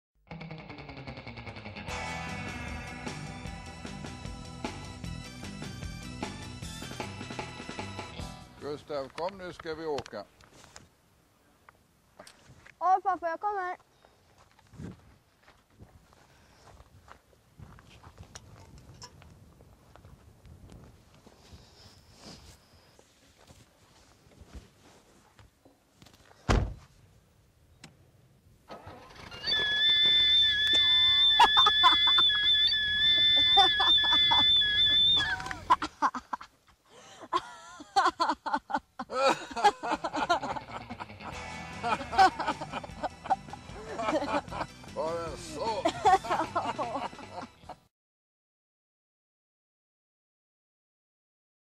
Avgastjut - Butterick's
En av Butterick's klassiska skämtartiklar. Avgastjut som får vilken bil som helst att låta helt annorlunda...